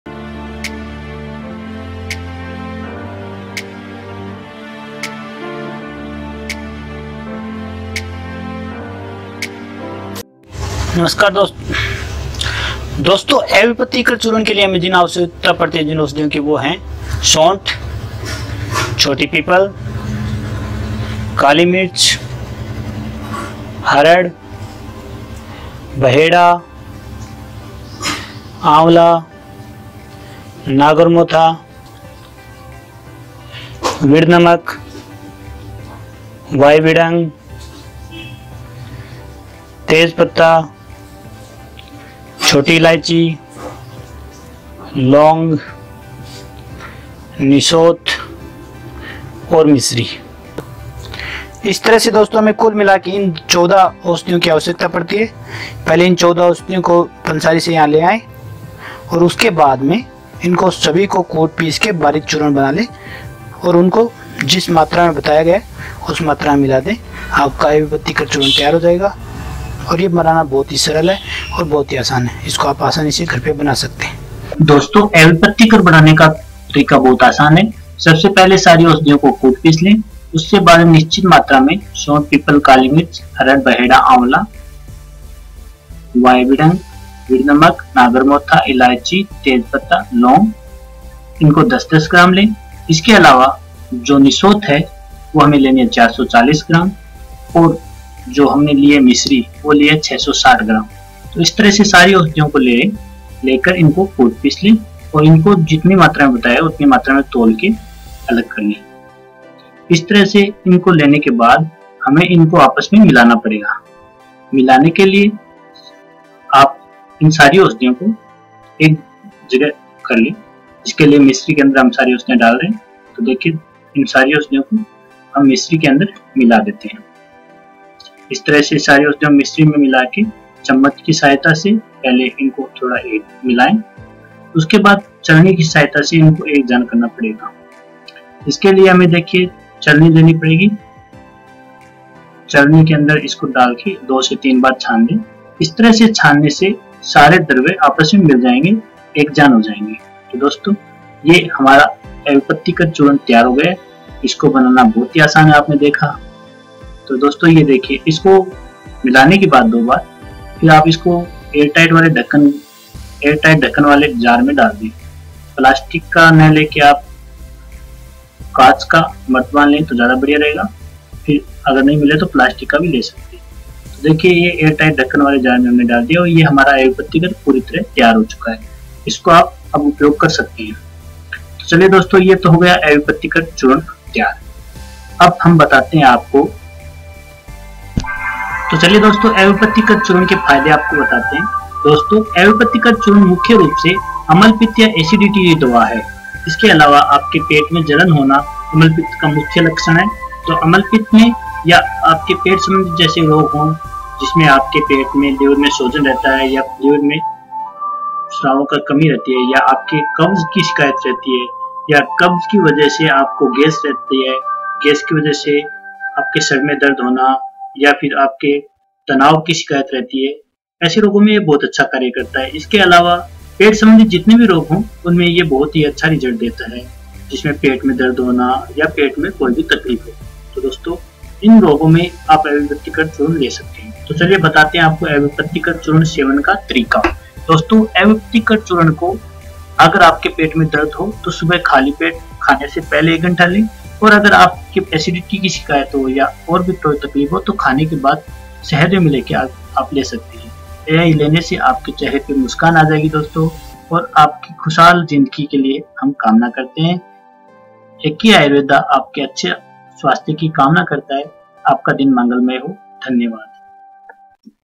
41.अविपत्तिकर चूर्ण अम्लपित्त की दवा बनाने का तरीका और उसके फायदे
अविपत्तिकर चूर्ण बनाना बहुत आसान  इसे आप घर पर बना सकते है |